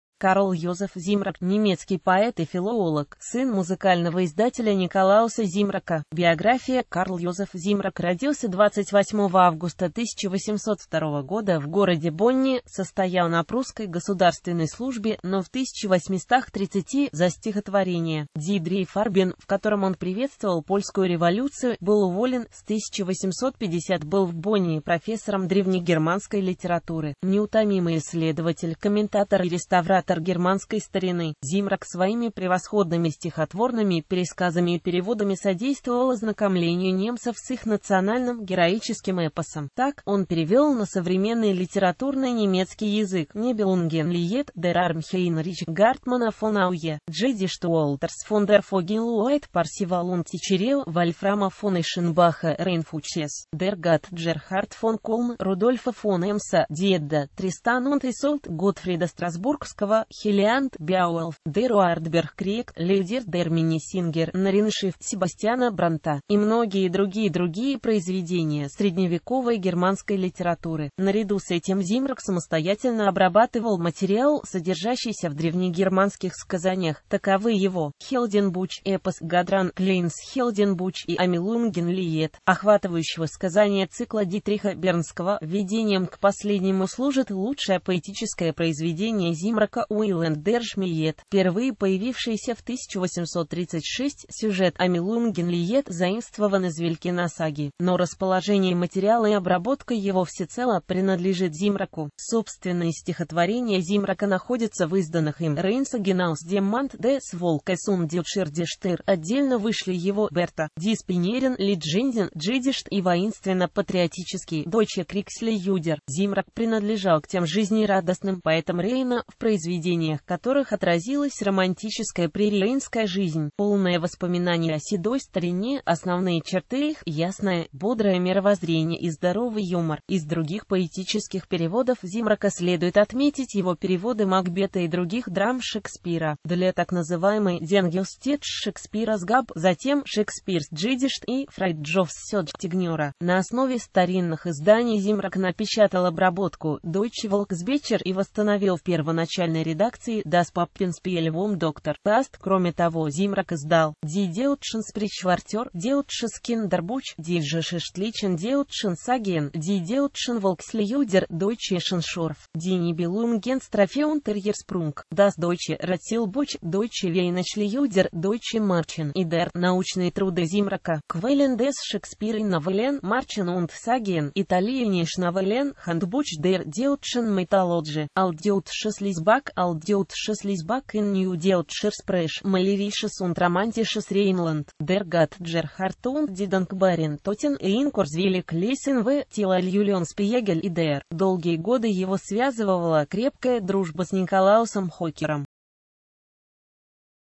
Зимрок, Карл Йозеф
Карл Йозеф Зимрок — немецкий поэт и филолог.Сын музыкального издателя Николауса Зимрока.